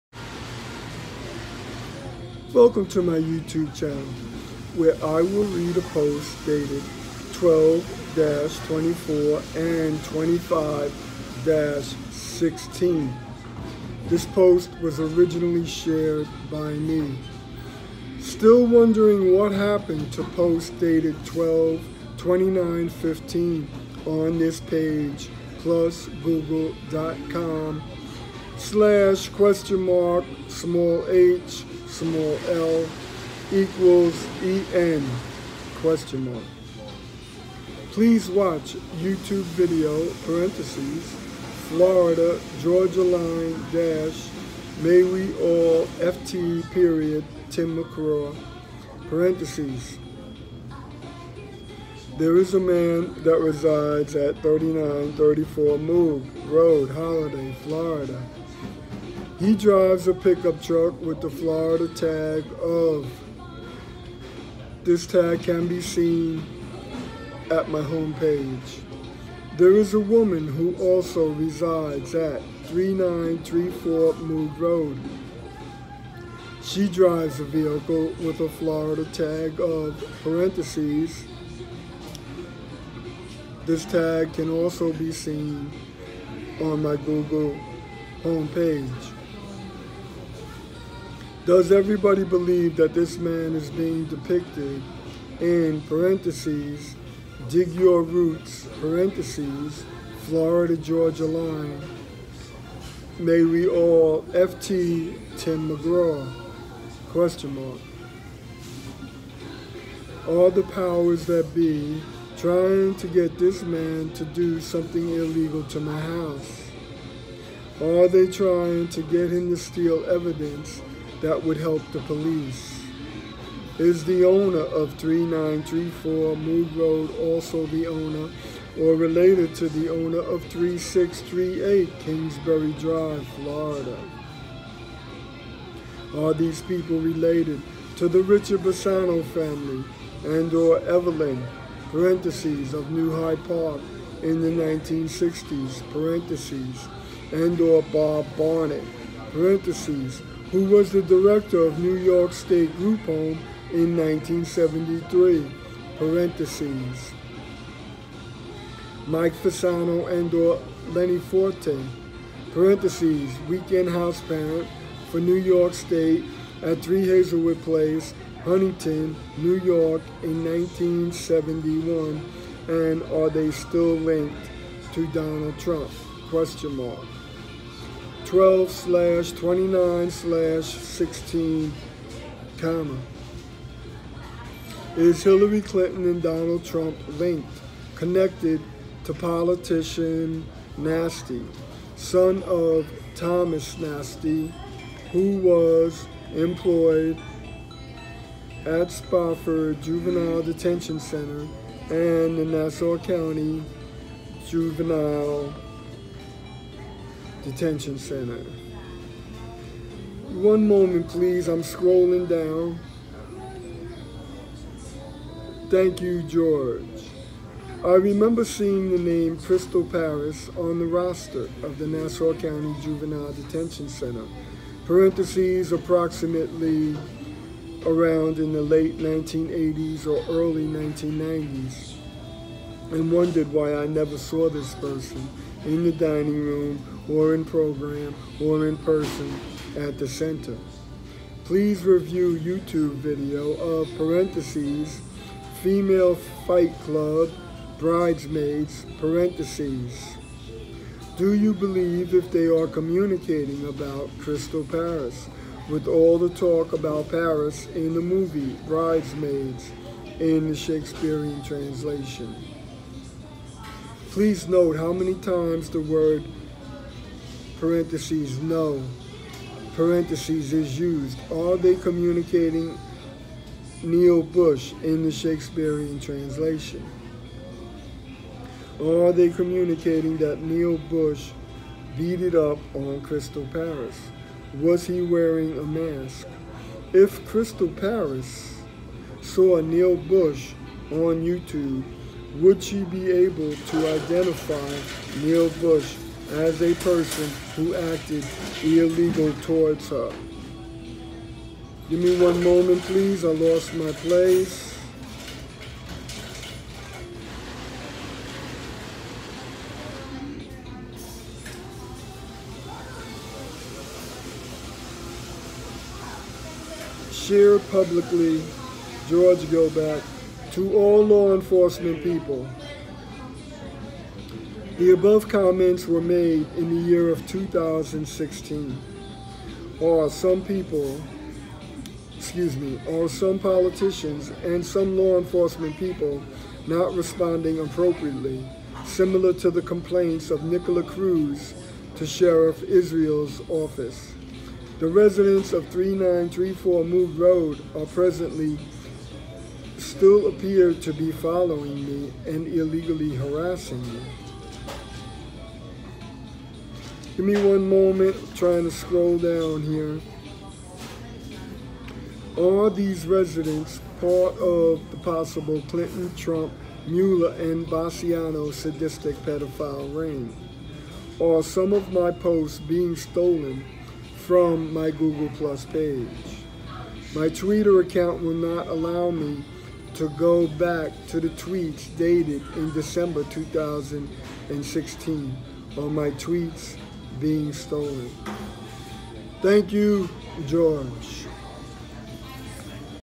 Is there a Post missing from my
Did Neil Bush rape under age Girls who worked at Show World owned by Richard Basciano ? Is Neil Bush communicated about (shakespearean) in the movie The Last Castle & The Bridesmaid ?